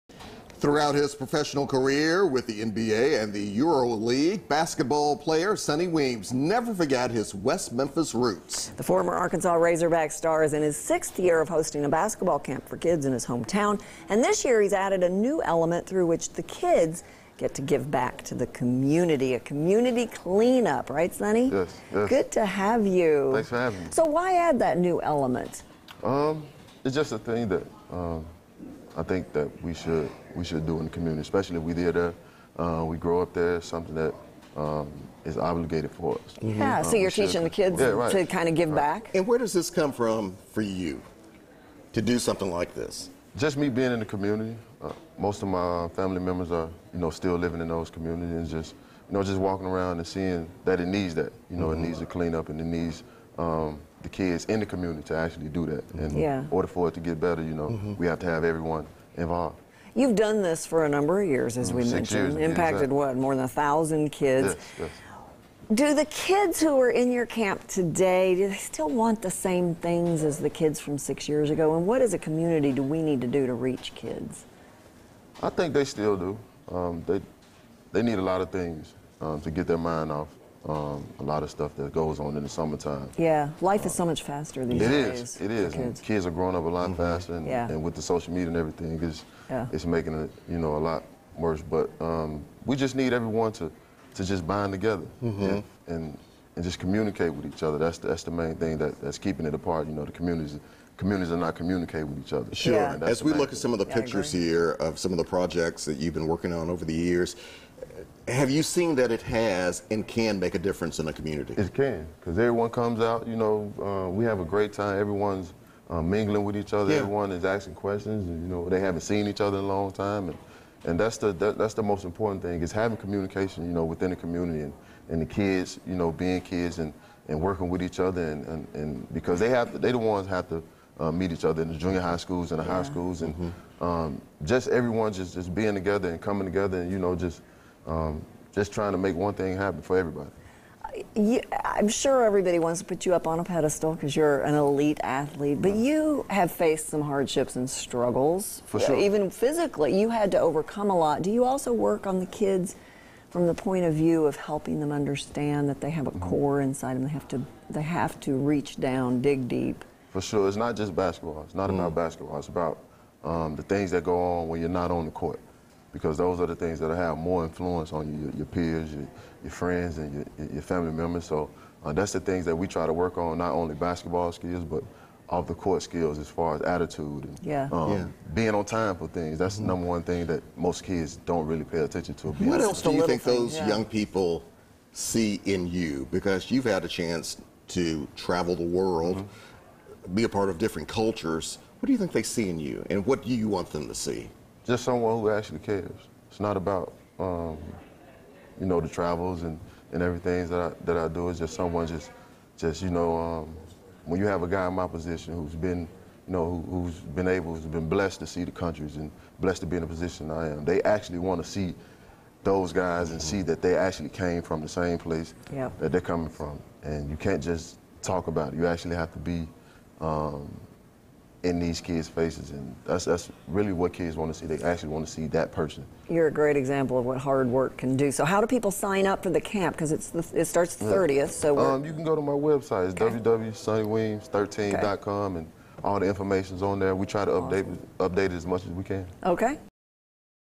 Basketball camp with Sonny Weems
Throughout his professional career with the NBA and the Euro-League, basketball player Sonny Weems never forgot his West Memphis roots. The former Arkansas Razorback star is in his sixth year of hosting a basketball camp for kids in his hometown and this year he`s added a new element, through which the kids give back to the community.